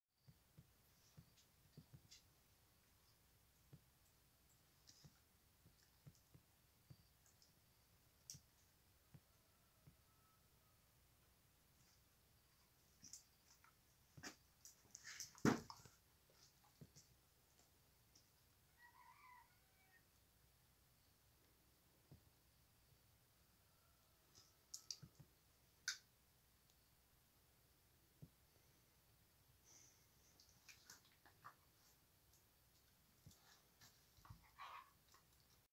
Where'd She Go?
Shazam and Hunter get more animated as they wake up more. Shazam tries to scratch himself with his back leg, but fails! hehehe.